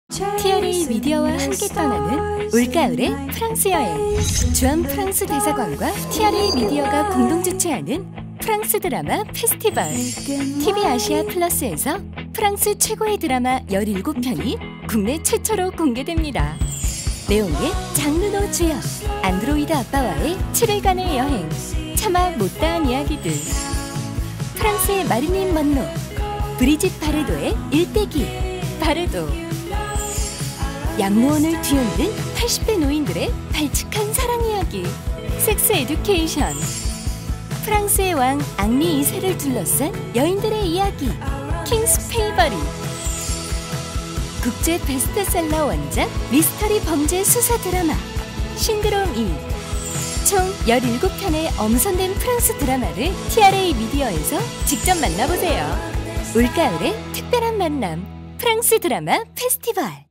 2023 프랑스 드라마 페스티벌
TRA Media와 주한 프랑스 대사관 문화과가 공동 주최하는 프랑스 드라마 페스티벌 (프드, FEDE)이 올 가을 여러분을 찾아옵니다! 9월 4일(월) 부터 10월 25(수)까지, 국내에 공개된 적 없는 고품격 프랑스 드라마 17편이 국내 최초로 TVAsia Plus(티브이아시아플러스)에서 방영됩니다. 자세한 편성 정보 및 채널 번호는 TRA Media 홈페이지 및 프랑스 대사관 문화과 홈페이지 확인해 주세요.

French Drama Festival (FEDE), co-organisé par TRA Media et le Service culturel de l'Ambassade de France en Corée, est lancé ! Du lundi 4 septembre au mercredi 25 octobre, 17 fictions françaises de grande qualité, seront diffusées pour la première fois sur TVAsia Plus. Pour obtenir des informations détaillées, veuillez consulter le site web de TRA Media et le site web du Service culturel de l'Ambassade de France